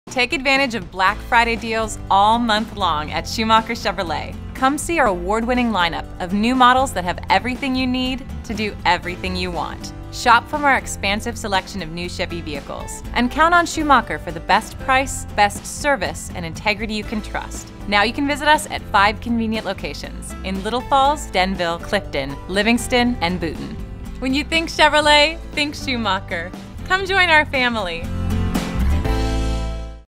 Black Friday Deals All Month Long at Schumacher Chevrolet Auto Group
Take advantage of Black Friday deals all month long at Schumacher Chevrolet! Come see our award-winning lineup of new models that have everything you need to do everything you want! Shop from our expansive selection of new Chevy vehicles and count on Schumacher for the best price, best service, and integrity you can trust. now you can visit us at 5 convenient locations in Little Falls (8 Main St.), Denville (281 Route 46 E.) Clifton (999 Route 46 E.), Livingston (606 Rt. 10 W.) and Boonton (650 Myrtle Ave.).. Visit us When you think Chevrolet, think Schumacher.